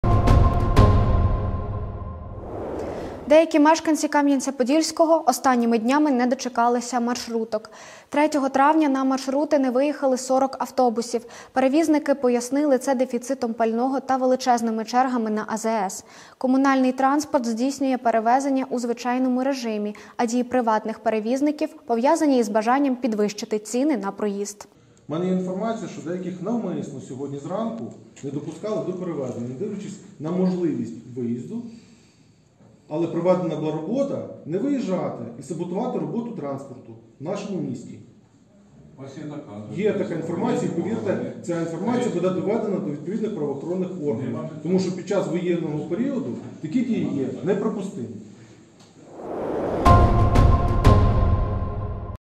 SOS: КУДИ ПОДІЛИСЯ МАРШРУТКИ? (05.05.2022)
Деякі мешканці Кам’янця-Подільського останніми днями не дочекалися маршруток.

‪#‎КамянецьПодільський‬ ‪#‎КаменецПодольский‬ ‪#‎KamyanetsPodilskiy‬